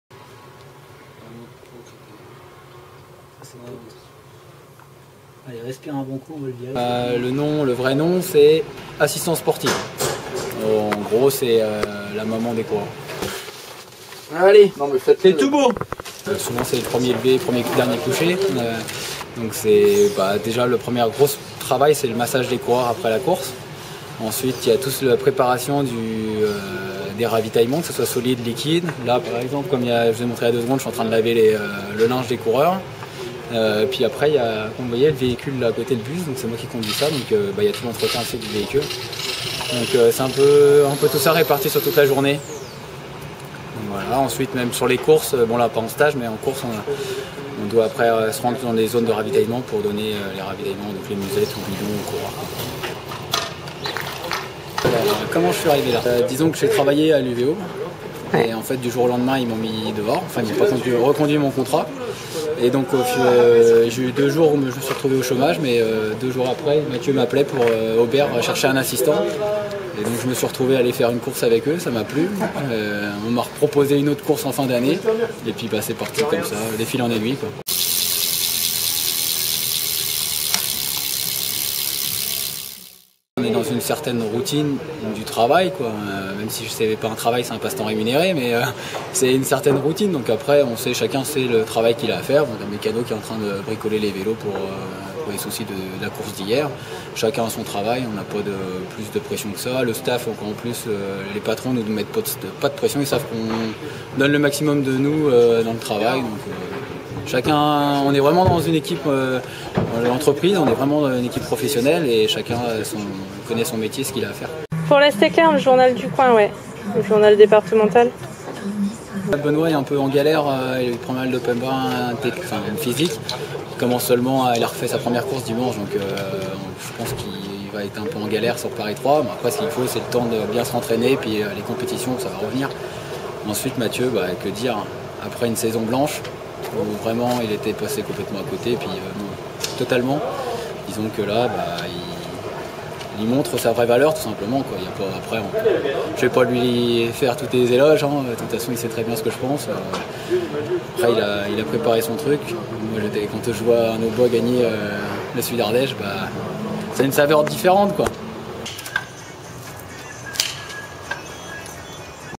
Paris-Troyes : rencontre avec la "maman" des coureurs de l'é
Rencontre avec Guillaume Corniau, "la maman des coureurs" de l'équipe BigMat Auber 93.